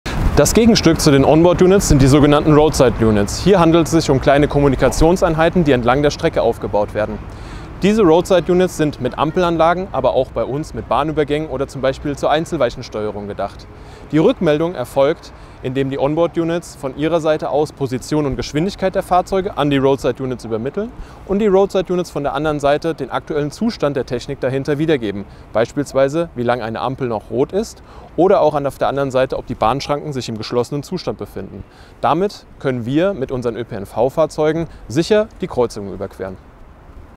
Frankfurt MIND(+): Die Road-Side-Units
Mit dem Verbundvorhaben Frankfurt MIND(+) schaffen die VGF und das Straßenverkehrsamt Frankfurt die Mobilität der Zukunft in der Mainmetropole und leisten einen wichtigen Beitrag zum Klimaschutz und zu einer lebenswerten Stadt.
Der innerstädtische Verkehrsfluss wird optimiert und der ÖPNV gestärkt. Dafür bauen die beiden Partner ein intelligent gesteuertes Verkehrsmanagement aus, erweitern das Angebot von Bus und Bahn und optimieren die Verknüpfung einzelner Fahrten innerhalb des ÖPNV sowie mit dem (motorisierten) Individualverkehr (MIV) – ganz im Sinne von Nachhaltigkeit und Umweltschutz.

Was "Road-Side-Units" sind und welche Rolle sie dabei spielen, erklärt VGF-Projektleiter Florian im Video.